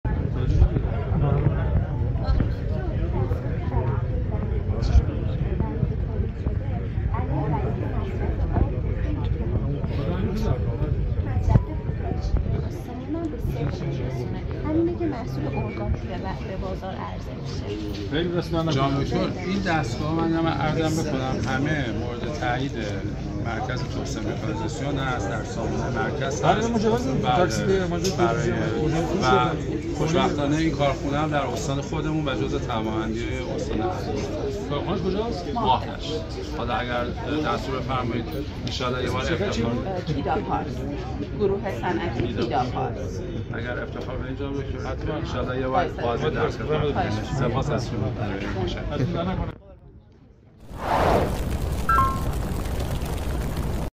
تیداپارس در نمایشگاه کشاورزی
حضور تیداپارس در نمایشگاه دستاوردهای کشاورزی، امنیت غذایی و زیستی سازمان جهاد کشاورزی استان البرز

دانشکده کشاورزی و منابع طبیعی دانشگاه تهران